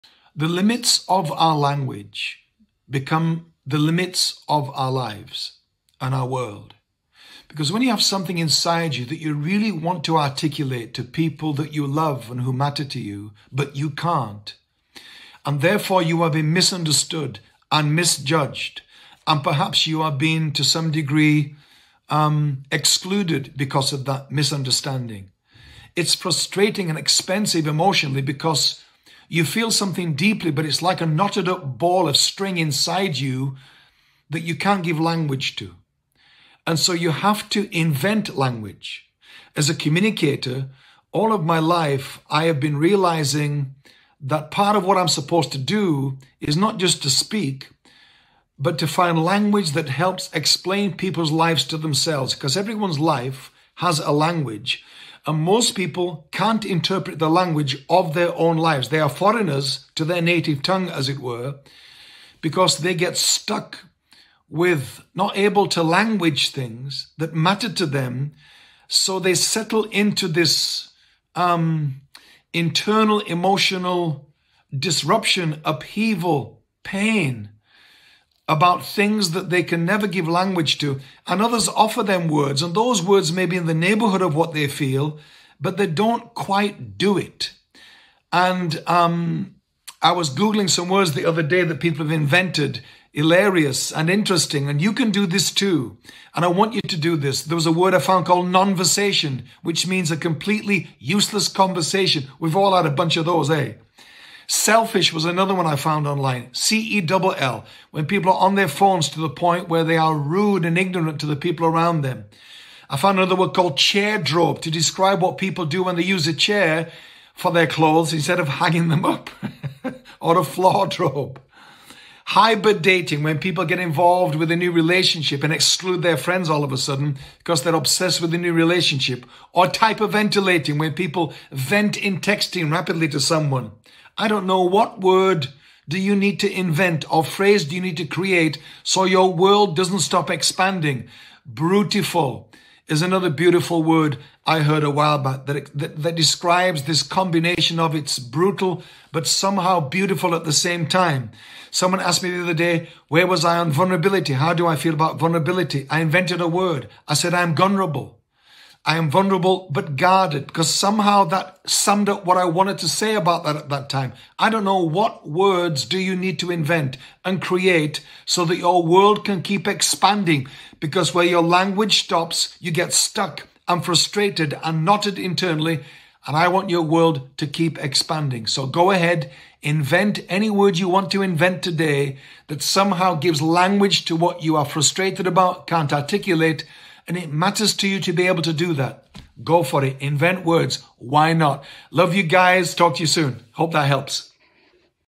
Why You Should Invent New Words
🗣️ Drop a comment with the word you will invent today and its meaning! 💬

The limits of your language are the limits of your life.
My job as a communicator is to give you language for your life.

I’m not just a speaker, I’m dealer in vocabulary, articulation and interpretation.

What word do you need to invent today? 📣